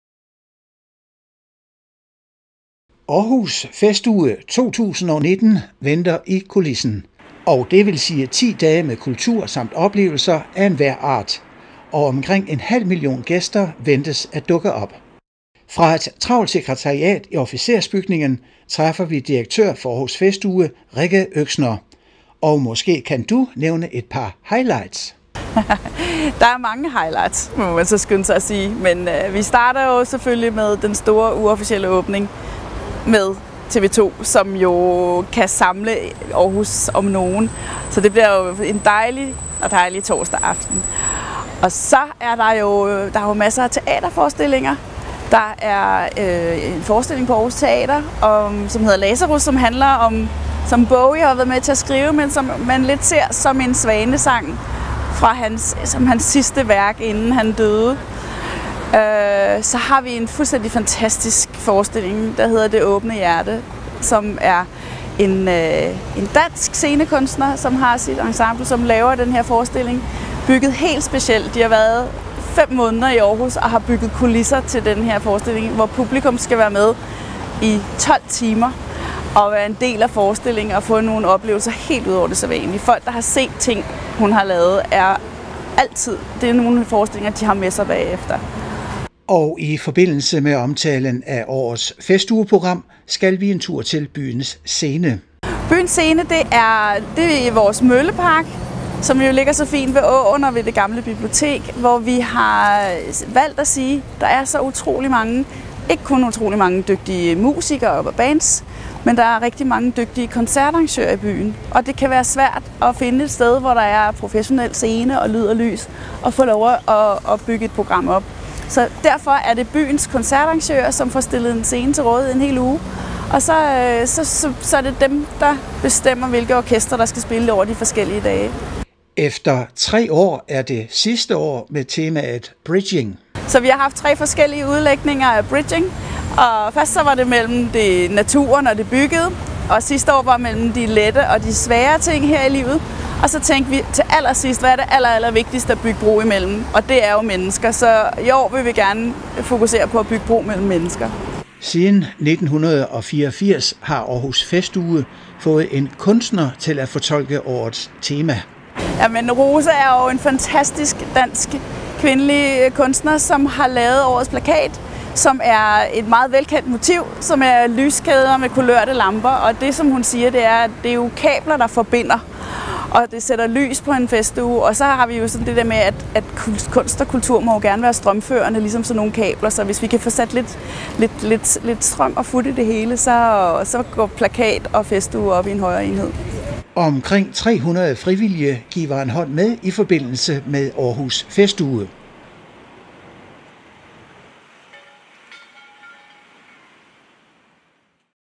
Rikke Øxner, Aarhus Festuge
Optaktsinterview til Aarhus Festuge 2019 med Festugedirektøren, Rikke Øxner